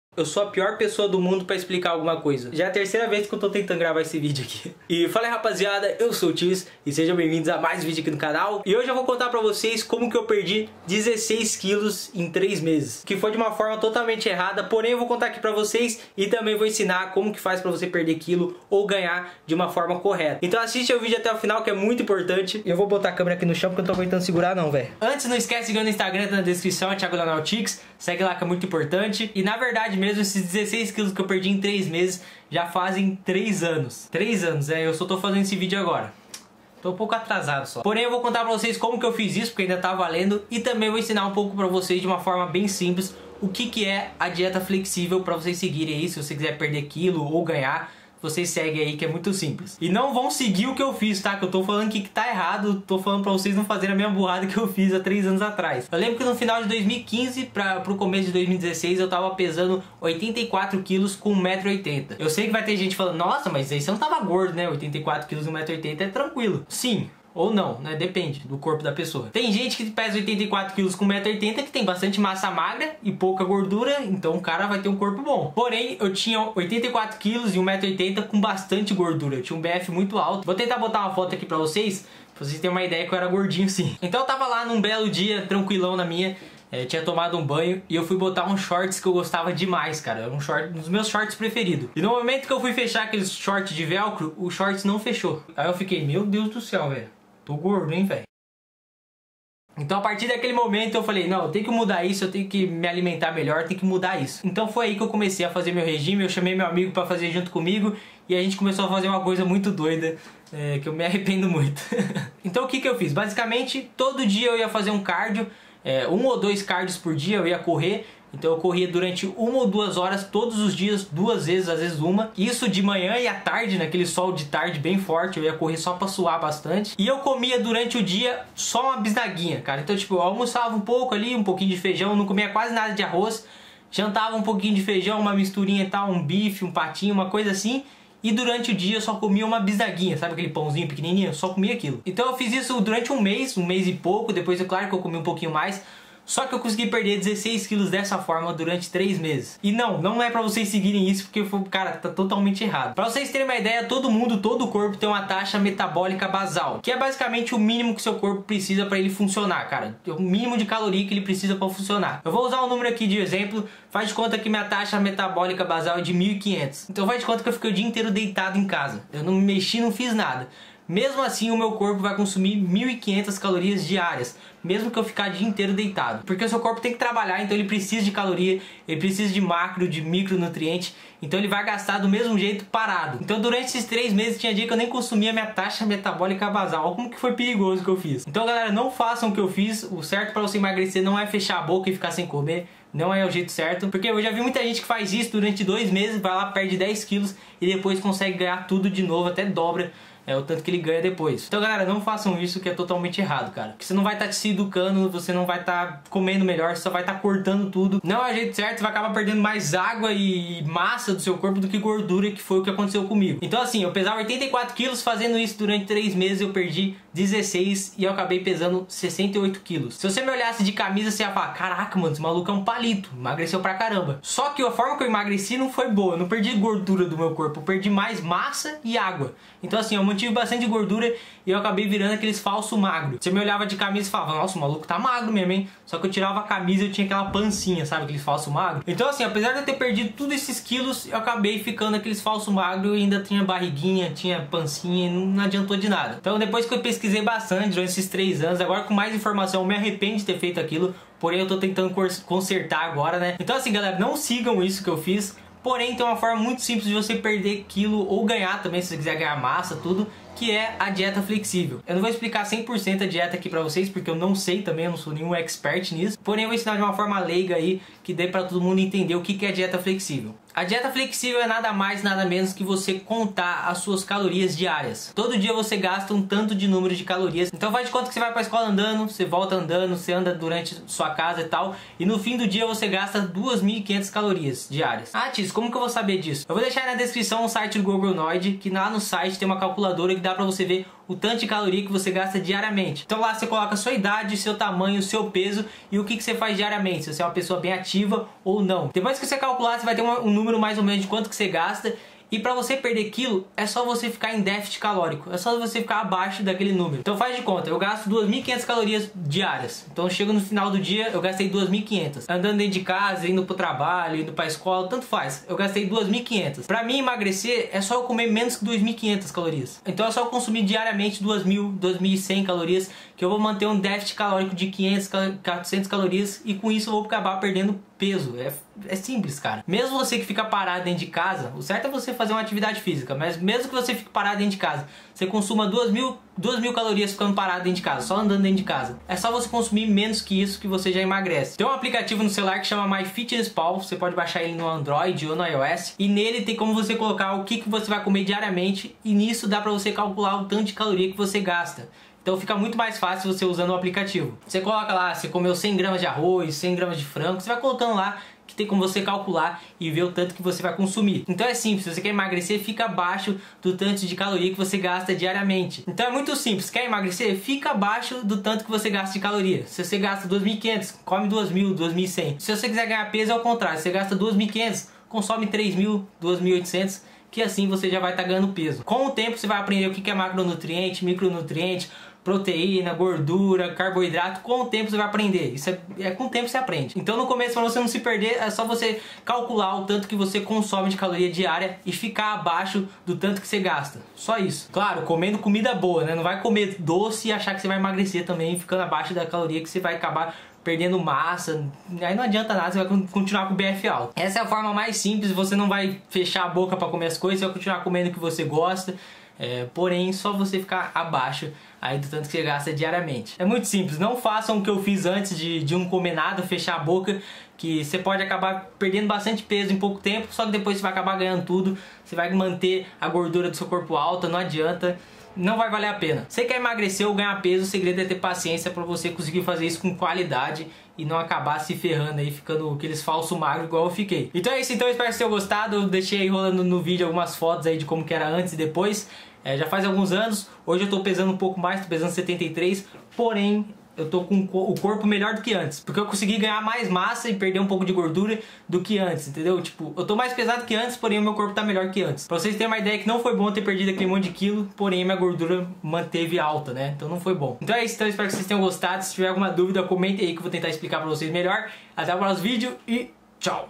COMO EU PERDI 16 KG EM 3 MESES ** Dieta Flexível **
Assista o vídeo todo para não fazer o que eu fiz kk e entender melhor sobre a dieta flexível.

◆Perguntas Frequentes

Sony Vegas

●Onde mora ?